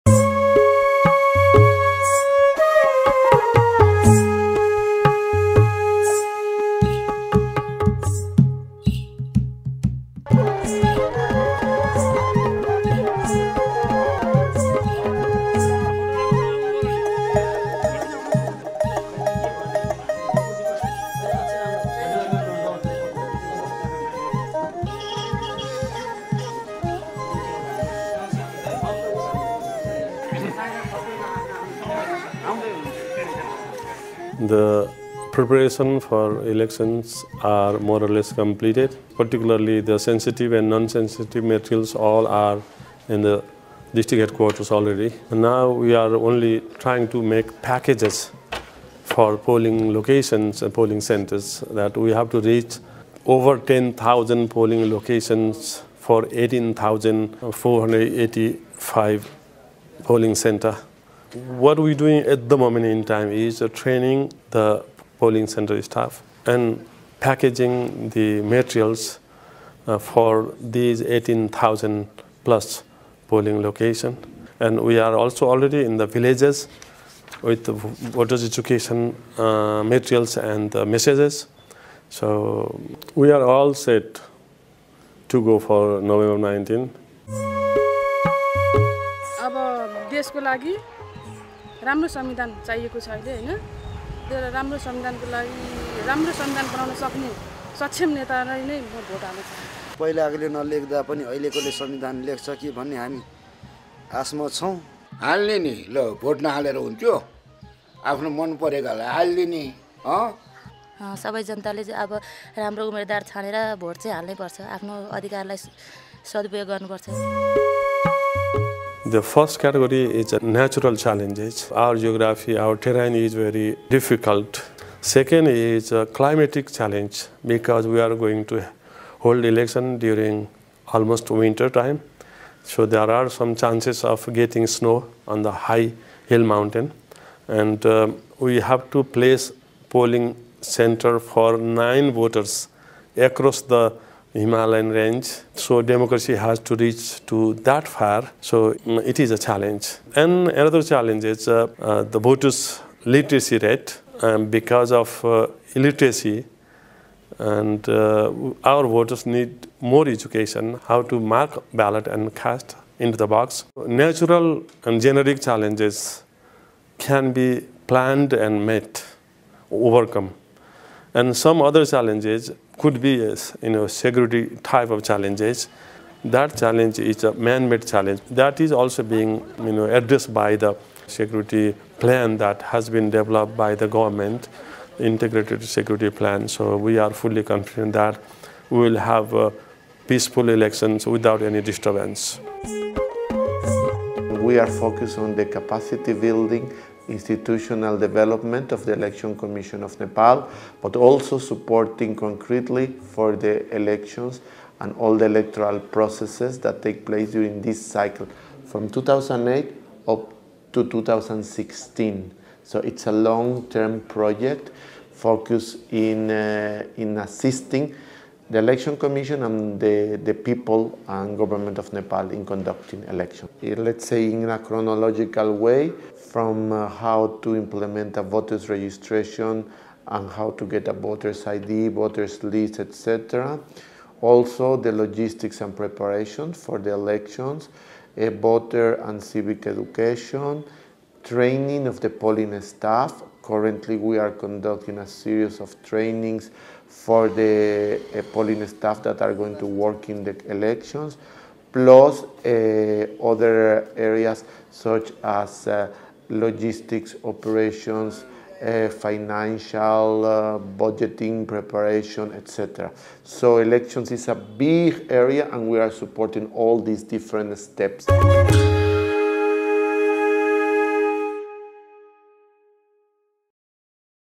Nepal Votes on 19 November, 2013
UNDP in collaboration with other development partners is supporting the Election Commission of Nepal to hold credible elections on 19 November, 2013